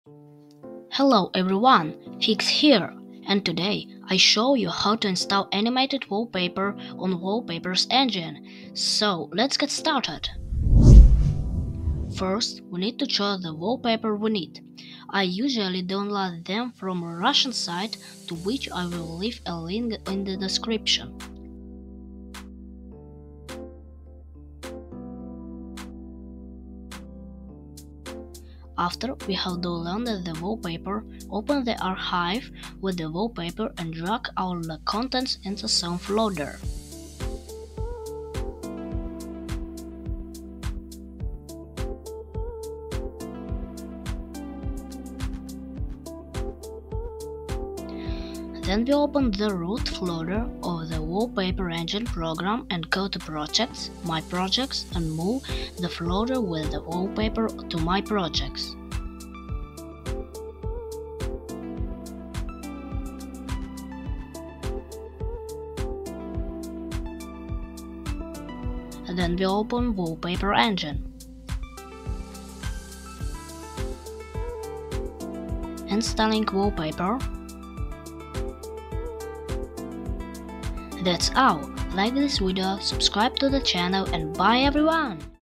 How to install animated wallpapers on Walpaper Engine! | Fiks Studios
In this video I will show you how to install animated wallpapers on Wallpaper Engine | Fiks Studios